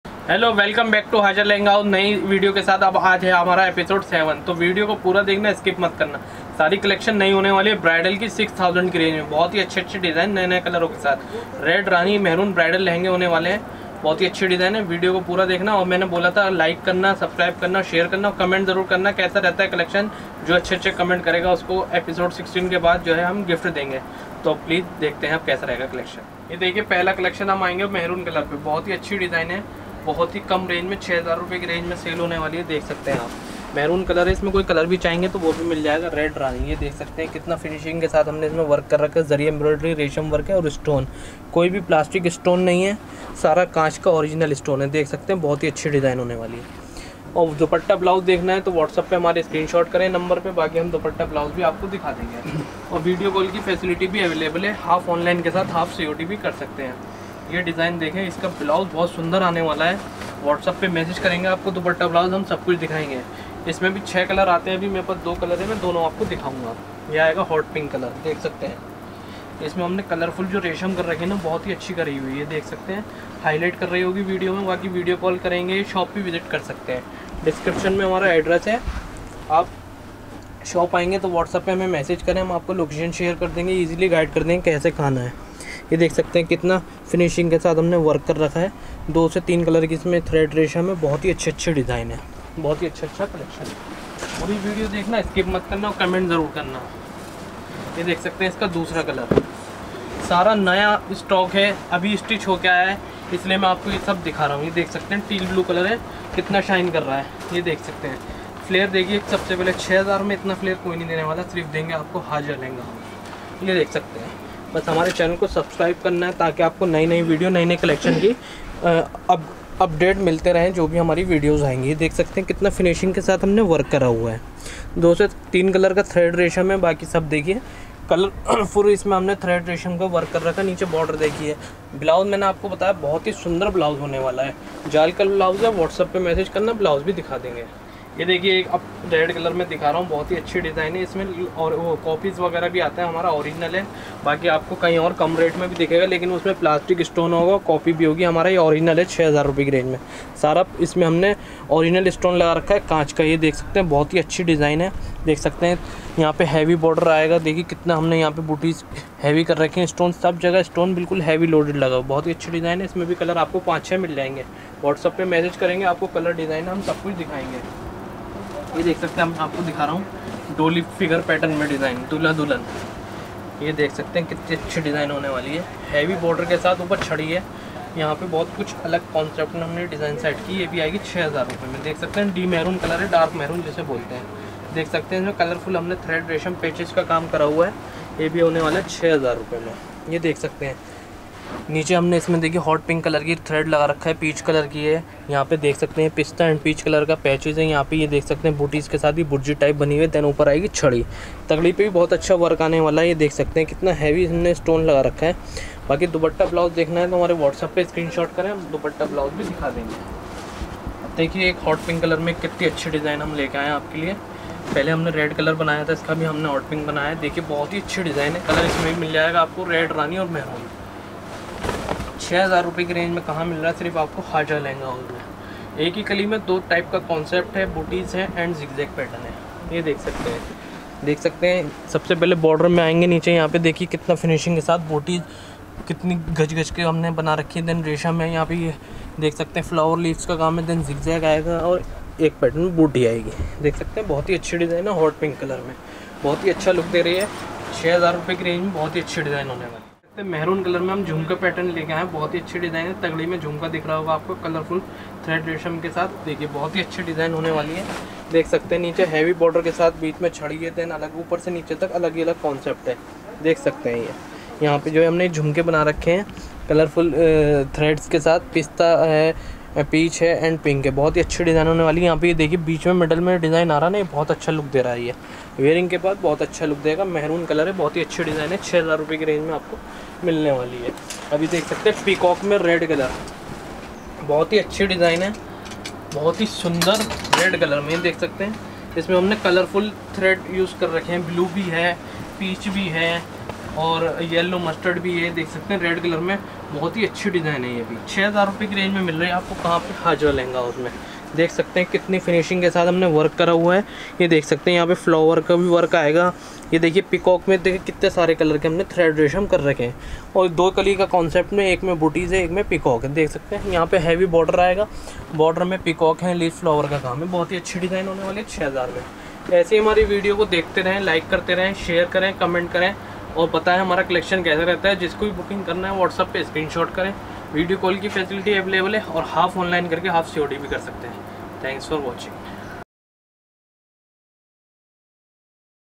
सस्ते दामो मे | Bridal Bright Lehanga in Chandni chowk | Designer Lehanga in Hajra Lehanga House
Hajra Lehanga House

Address - 5803 , Kucha Rehman , Ballimaran , Chandni Chowk , Delhi - 6